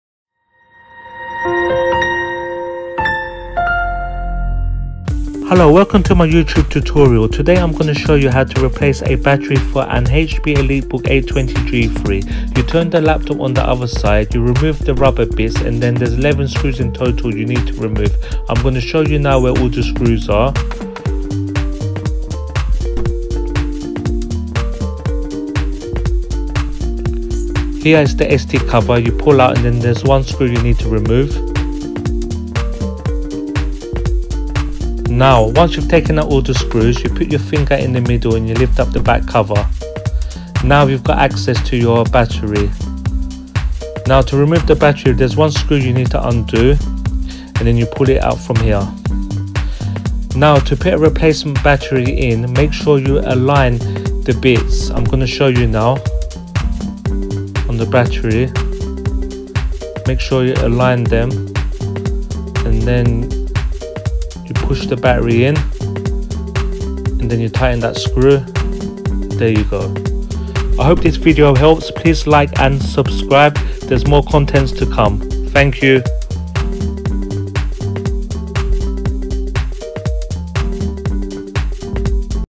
HP ELITEBOOK 820 G3 G4 BATTERY REPLACEMENT | substituição de bateria HP 820 G3 G4
Welcome to FIXtastic Contents.

This tutorial will show you how you can replace the battery of your HP Elitebook 820 G3/ G4 laptop.